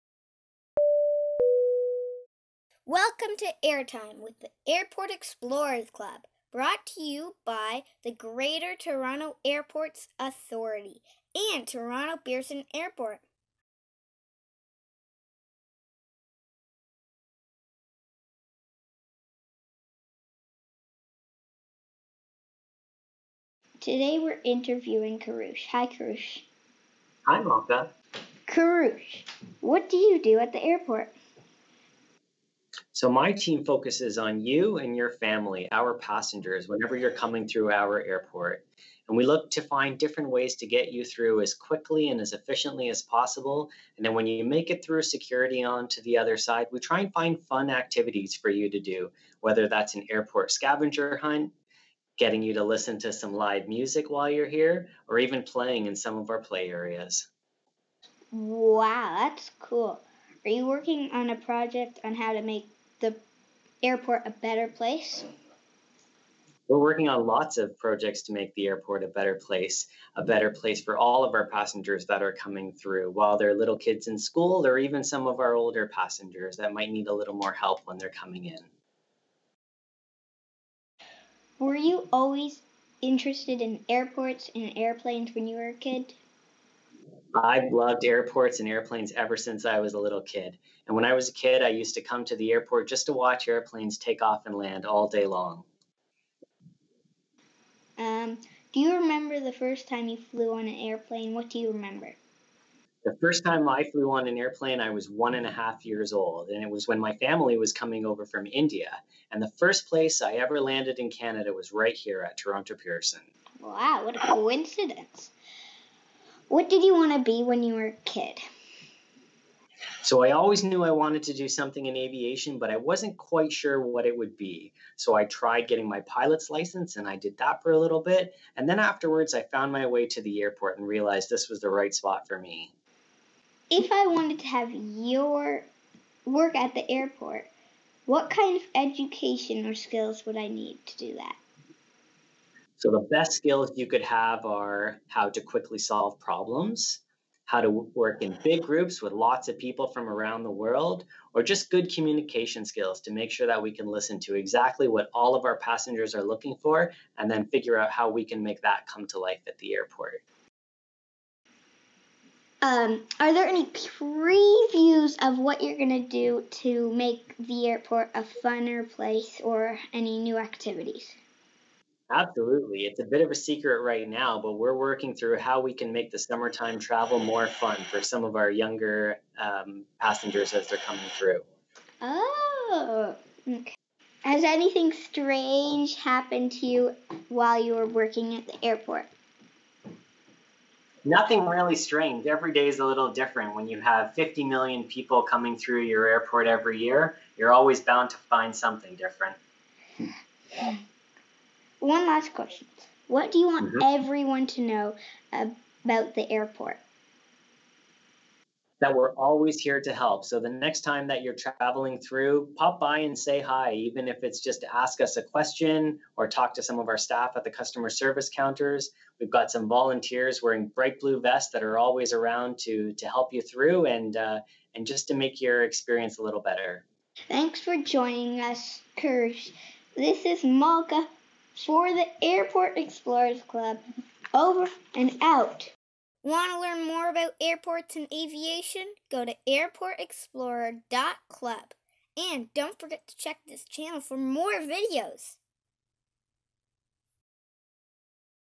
Airport Explorer Air Time: Kurush Minocher
Meet the person behind scavenger hunts, live music, and play areas at Toronto Pearson! Airport Explorer Malka interviews Director of Passenger Experience Kurush Minocher to find out what his team is doing to make the airport a fun and exciting place for passengers of all ages!